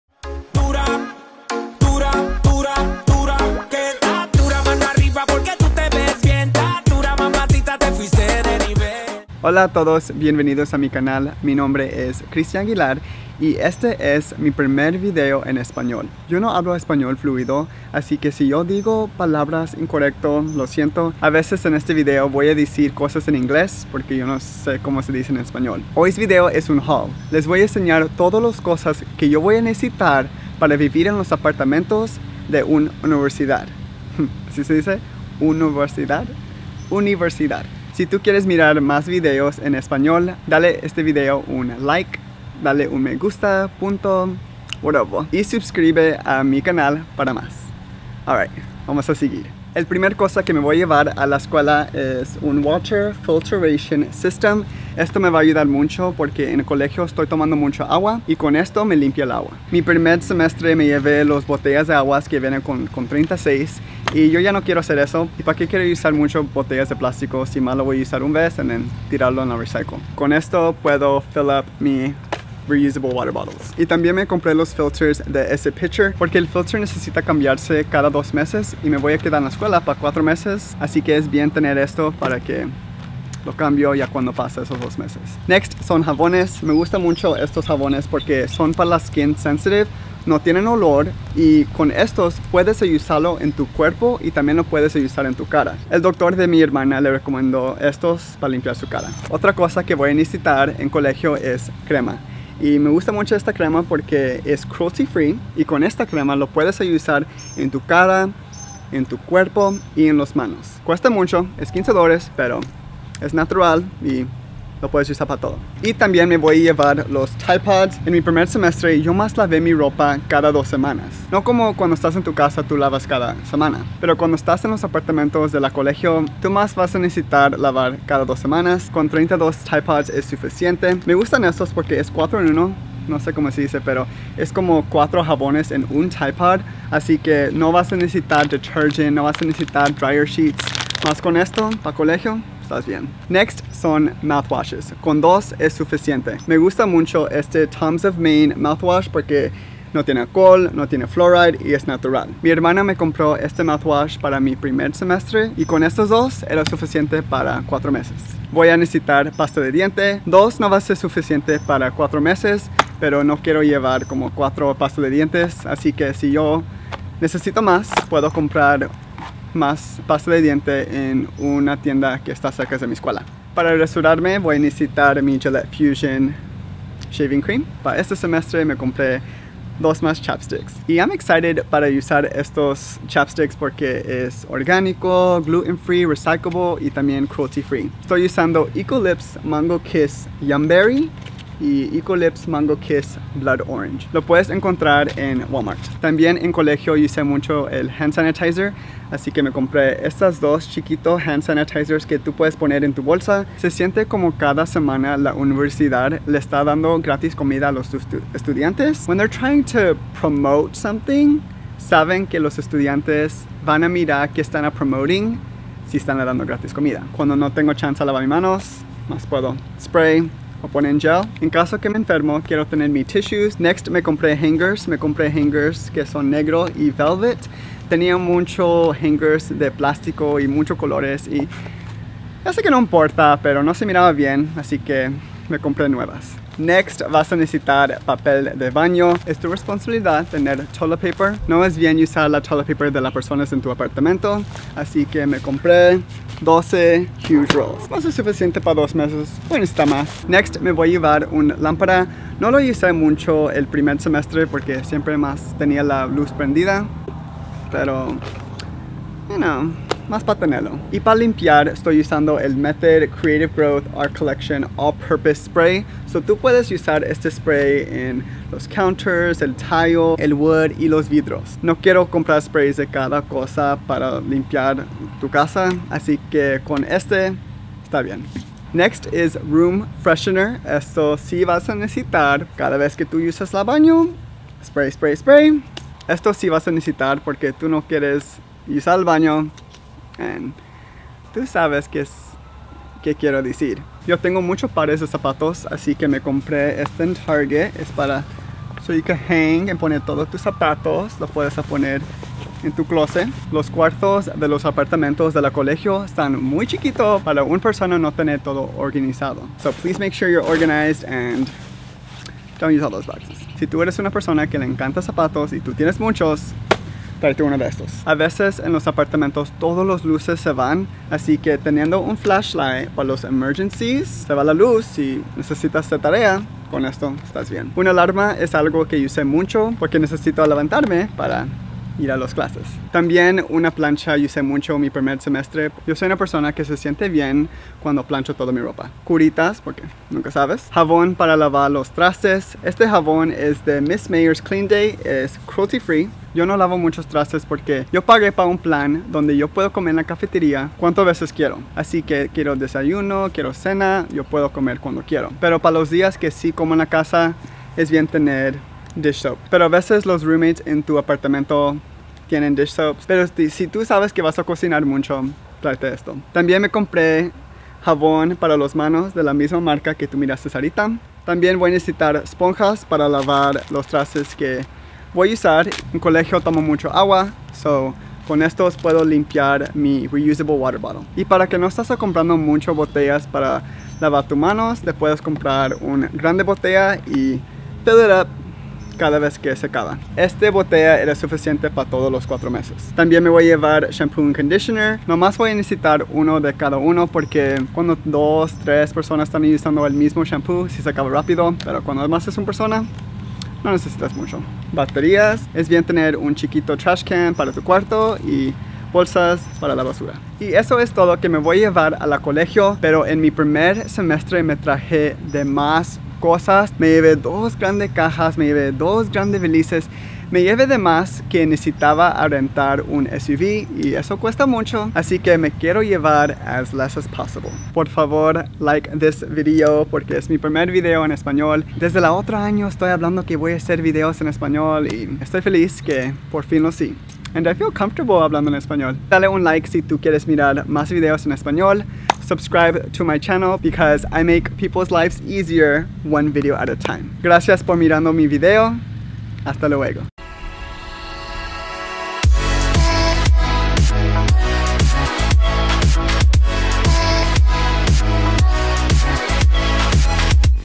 Lo Que Necesitas en tu Dormitorio de Universidad | My First Video in Spanish
Hola, este es mi primer video en español. En el video de hoy les muestro las cosas necesarias que todo estudiante colegial debe tener en su dormitorio.

También hago videos en inglés así que si están interesados, subscríbanse a mis otros canales.